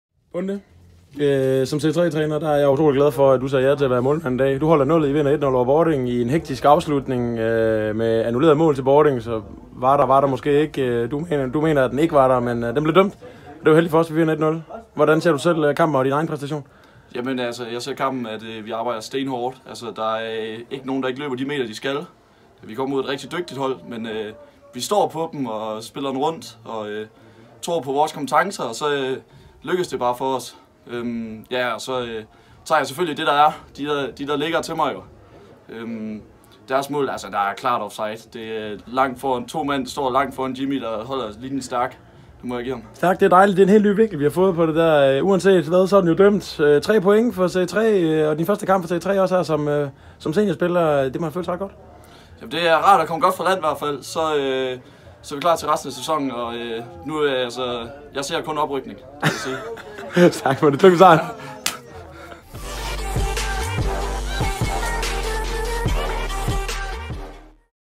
Magnus Bonde blev kampens spiller på Serie 3 !
Bonde skulle egentlig have spillet Serie 4 i aften, men lidt keeper mangel gjorde at han blev spurgt om han ville tage handskerne på Serie 3. Svaret var JA og Magnus kvitterede med 7 stemmer til kampens spiller, som han vandt, et clean sheet og en SUPER kamp 😀

Vi talte med Bonde efter kampen ! Se med her:

FORZA FREMAD 💙